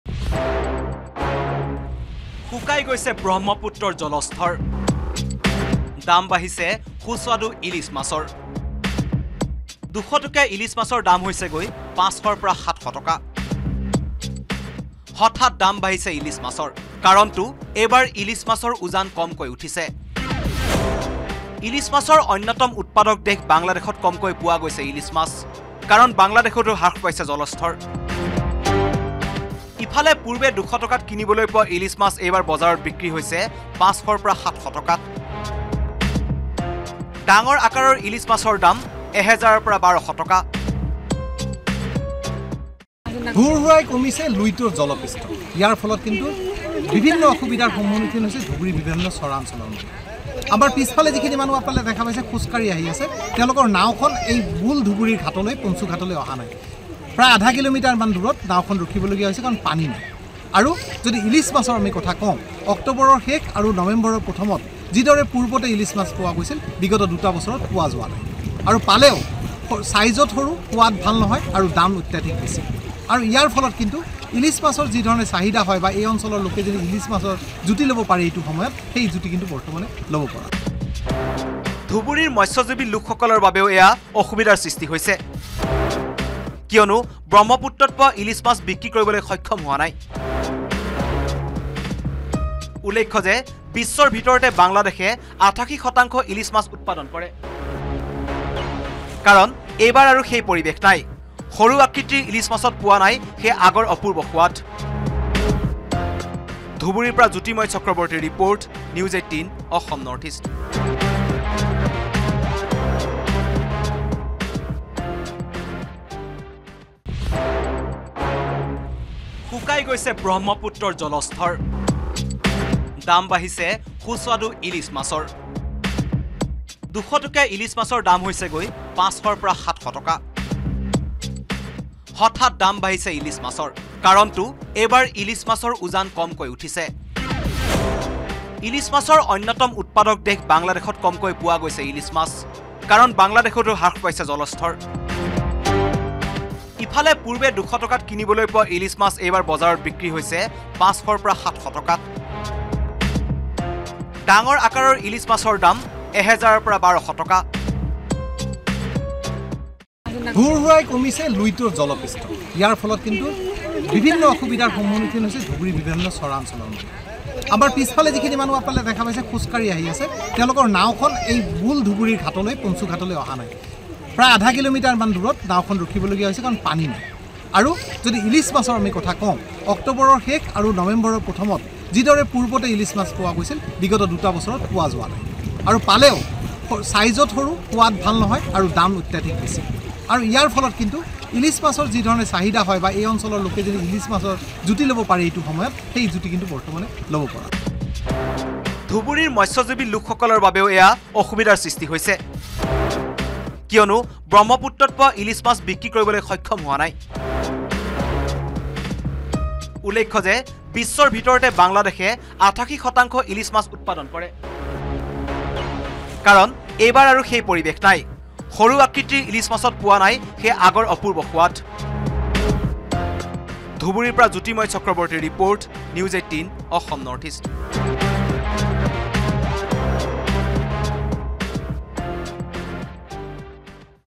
Hilsa Fish price Rise : সুস্বাদু ইলিছ মাছৰ দাম বৃদ্ধি পাইছে হঠাৎ | N18V
Hilsa Fish price Rise | আলু পিঁয়াজৰ পিছত এইবাৰ সুস্বাদু ইলিছ মাছৰ দাম বাঢ়িছে। ভাৰত বাংলা সীমান্তৰ ধুবুৰীত দাম উঠিছে ইলিছ মাছৰ। প্ৰতিকেজি ইলিছ মাছৰ দাম পূৰ্বৰ তুলনাত বৃদ্ধি হৈছে তিনিগুণকৈ। ইয়াৰ কাৰণ জানিলে আচৰিত হ’ব।